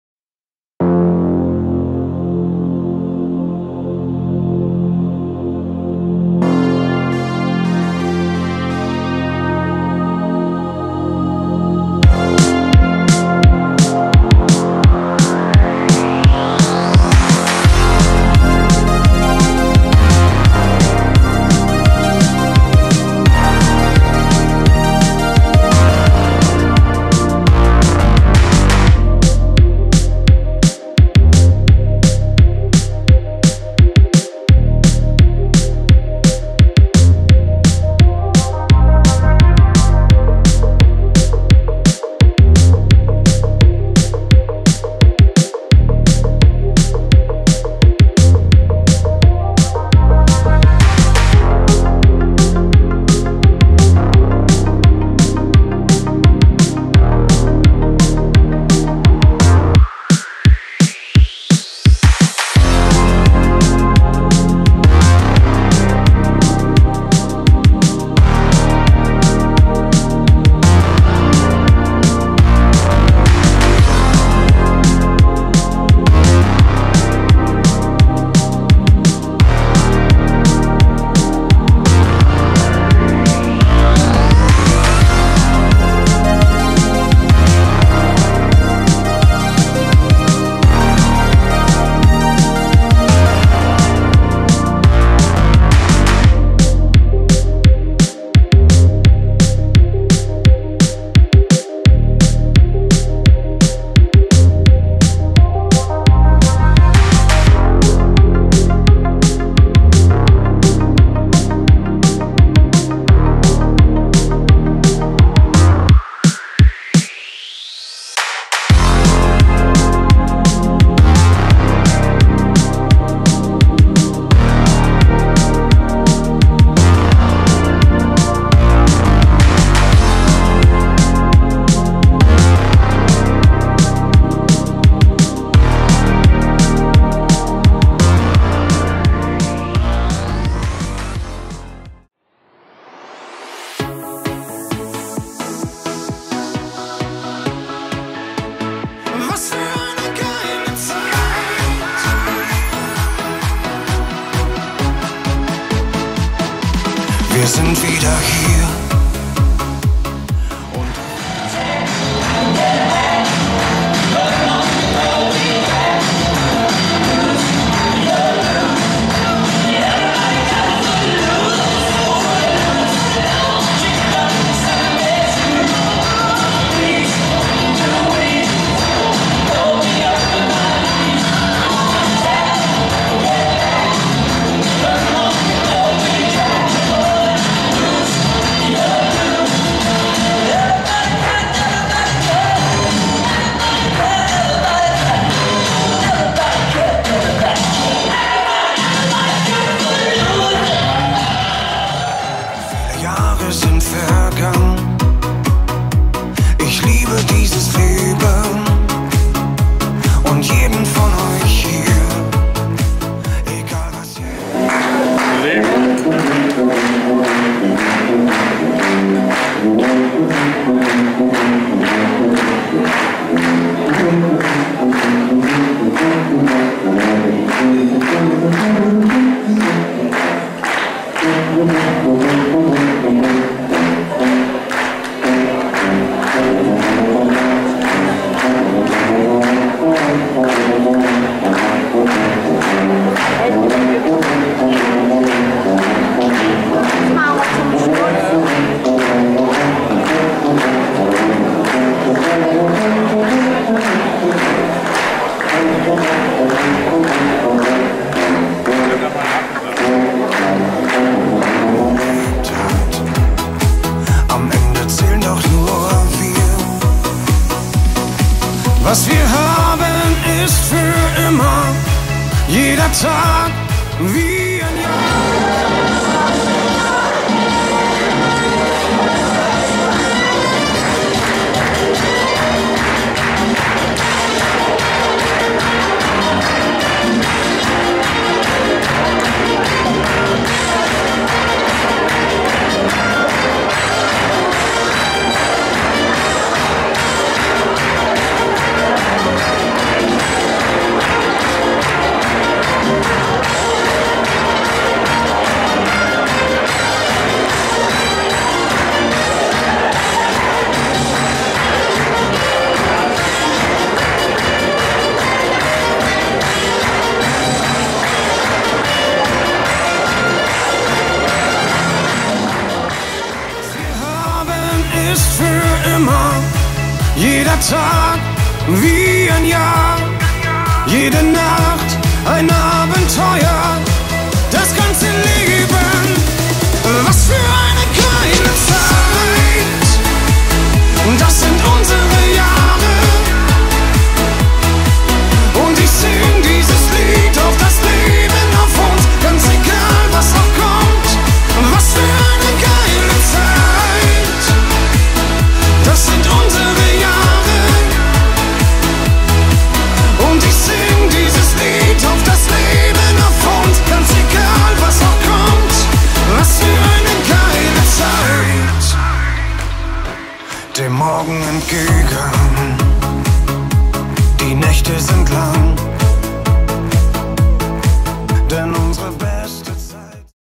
Throwback Rosenmontag 2020 Movie
Kleiner Rückblick unseres letztjährigen "Der Bär steigt in die Bütt" der Bärenzunft Bermatingen. Wir sind schon in voller Vorfreude auf die kommende Fasnet. Bleibt gesund.

Narri -- Narro